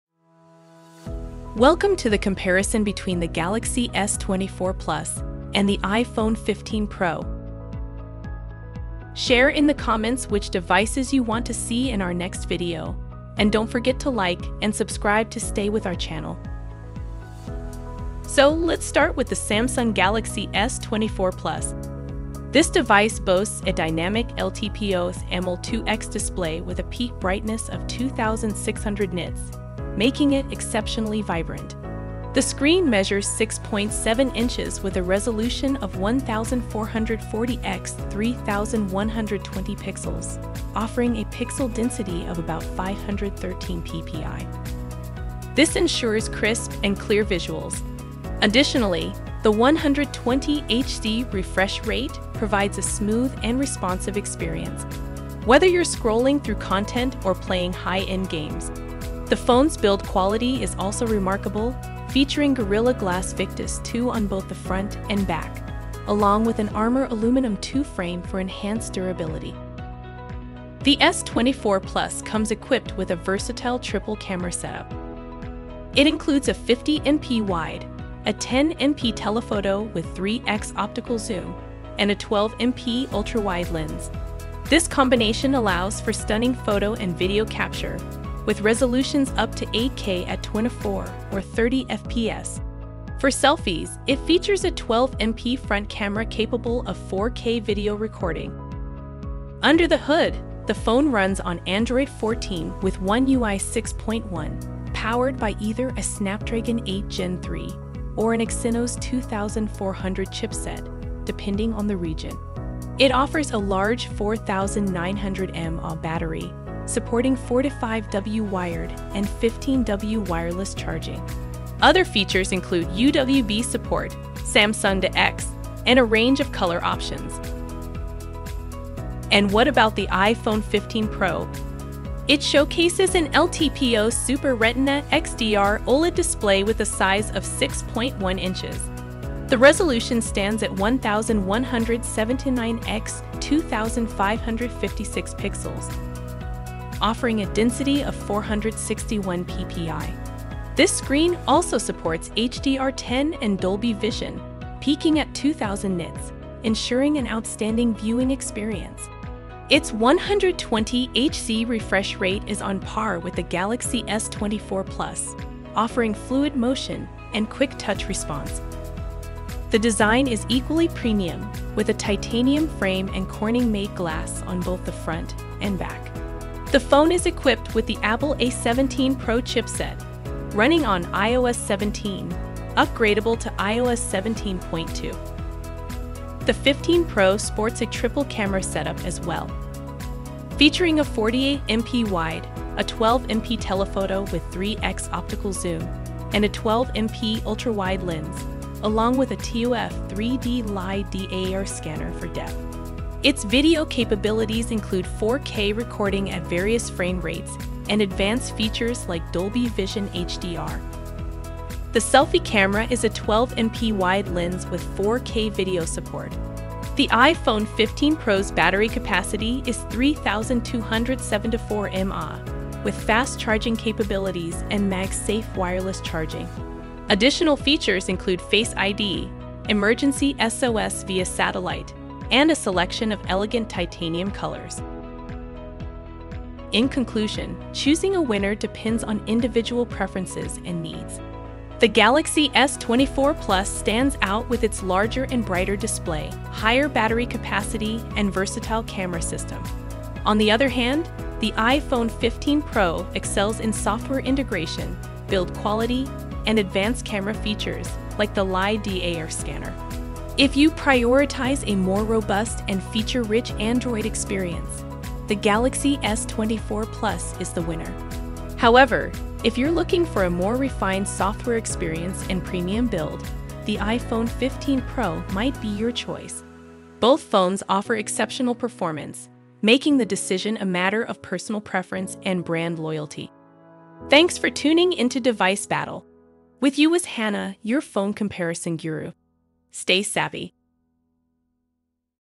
Samsung Galaxy S24 plus vs Apple iPhone 15 Pro | SMARTPHONE BATTLE with AI
Full specs comparison on APPLE iPHONE 15 PRO  vs SAMSUNG GALAXY S24 PLUS

Also don't forget about our website, where more devices and reviews are waiting for you

Compare.Decide.Enjoy!